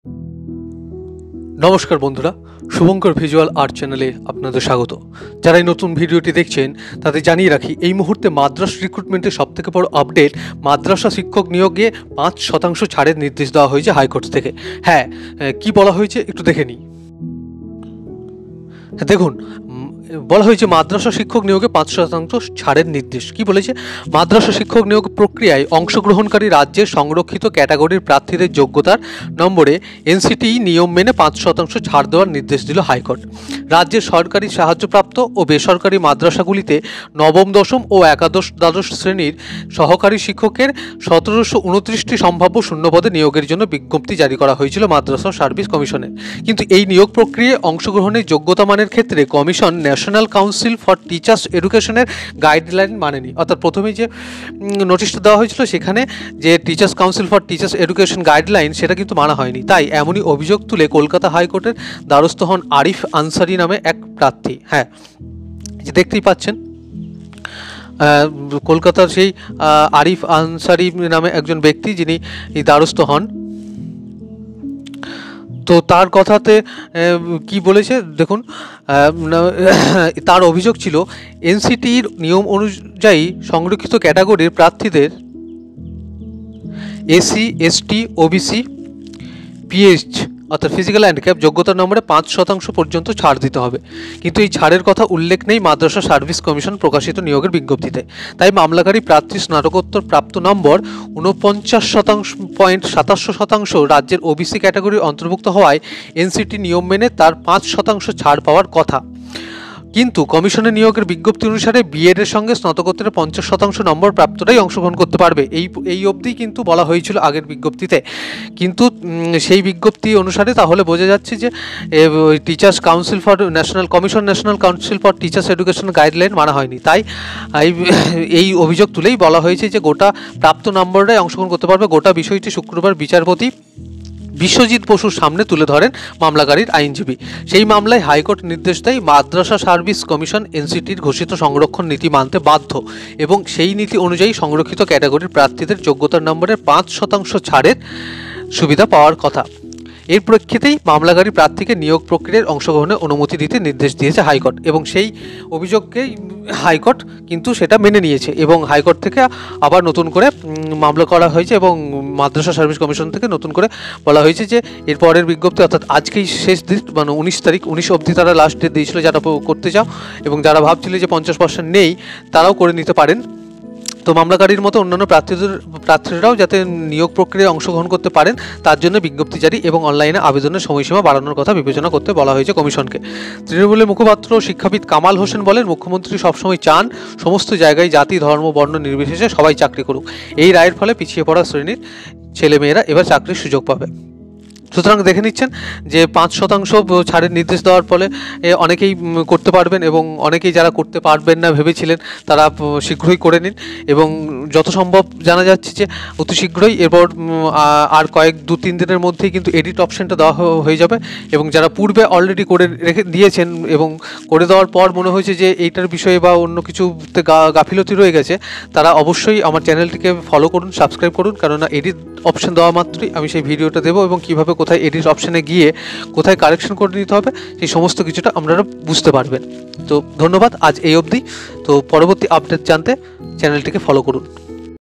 Madrasa service commission new update।। court order।। ৫শতাংশ ছাড় নম্বরে
Madrasa service commission recruitment 2023।। Form fill up 5% relaxation  in graduation marks।।